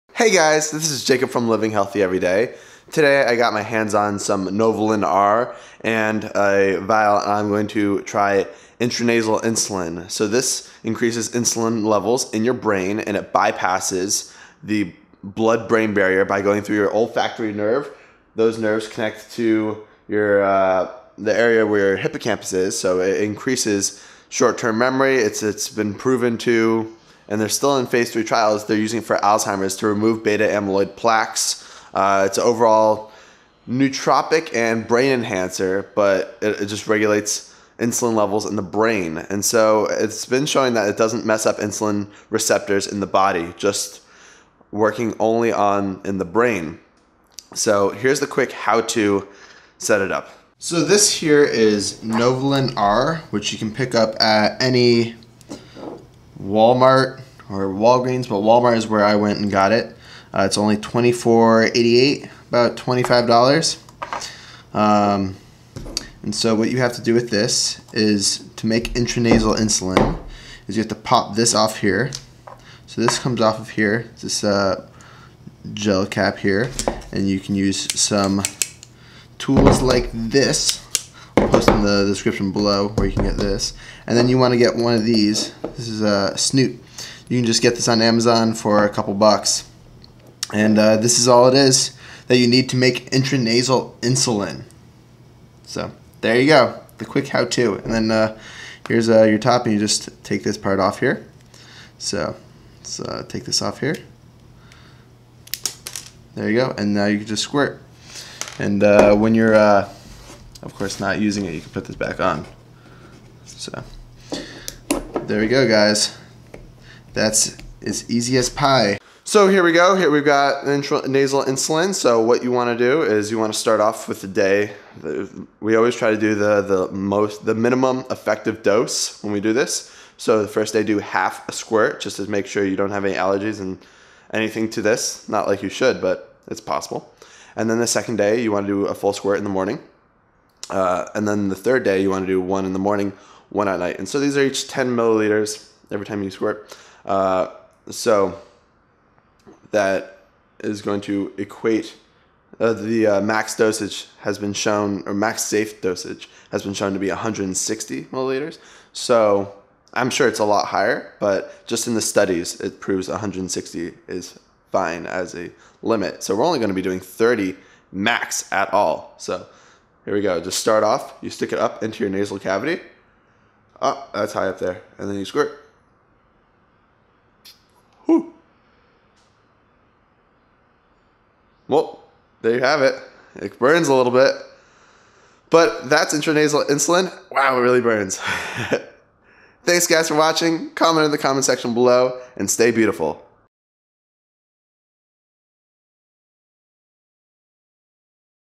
How to make INTRANASAL insulin - INI is a cheap brain BOOSTER
This is experimental and you should consult your healthcare professional before trying anything.

Here is the equipment I use:
Camera

Stay Beautiful!